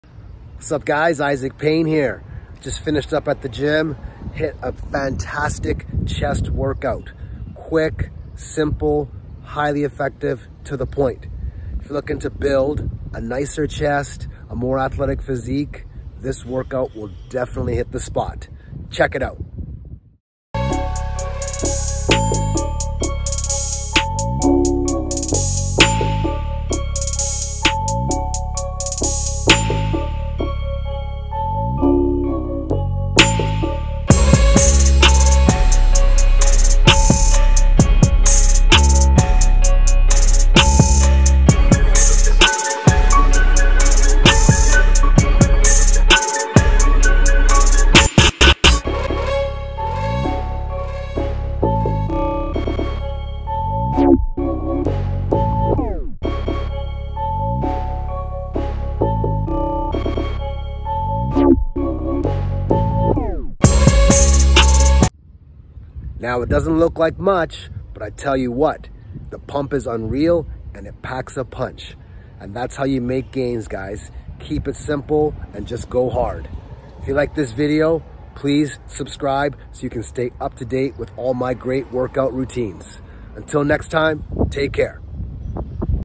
If you want a chiseled chest, this chest workout will do the trick!
If you want to build a chiseled chest, this chest workout will do the trick!

Isaac Payne doses a dumbbell chest workout that seems simple but is very effective for building a bigger chest.

Watch this video to see how you can build a nice chest in no time!